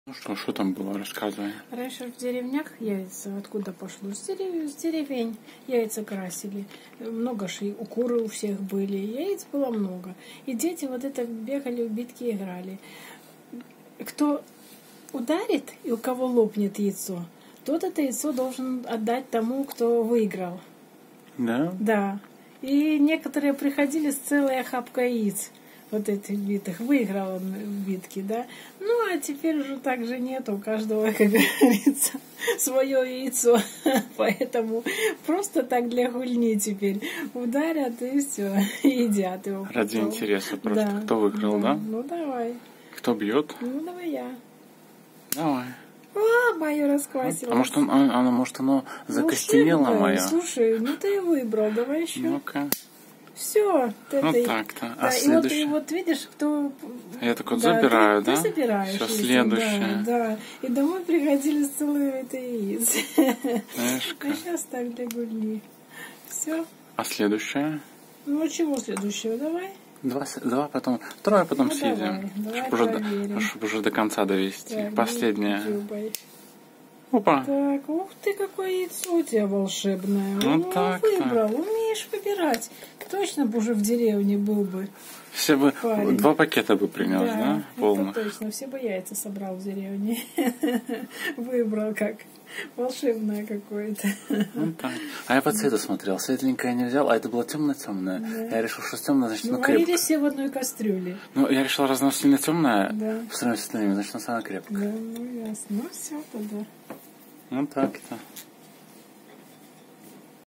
История происхождения игры в битки. Откуда взялась битва яйцами.
Мне рассказали откуда взялась эта игра в битву яйцами. А я решил снять это, чтобы и вы тоже знали :)